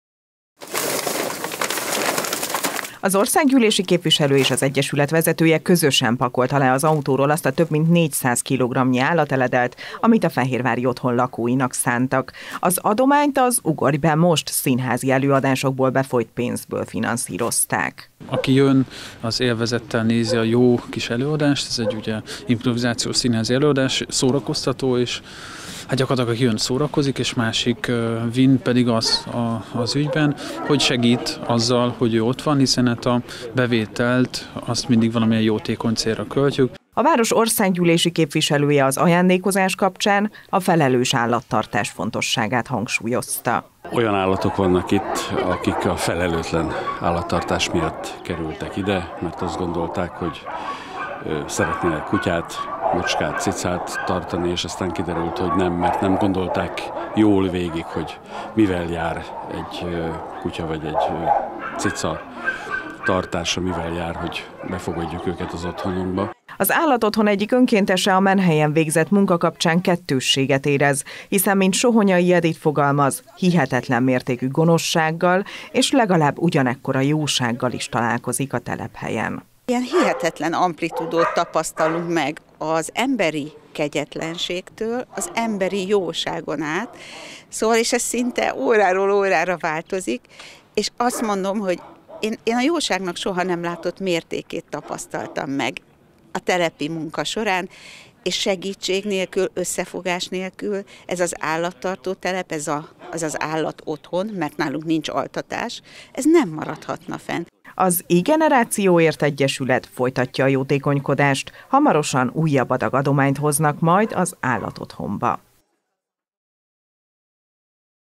Adomány a HEROSZnak
Újabb csomag adomány érkezett a HEROSZ Fehérvári Állatotthonába. Az iGenerációért Egyesület fehérvár országgyűlési képviselőjével közösen adta át az összegyűjtött kutya-és macskaeledelt.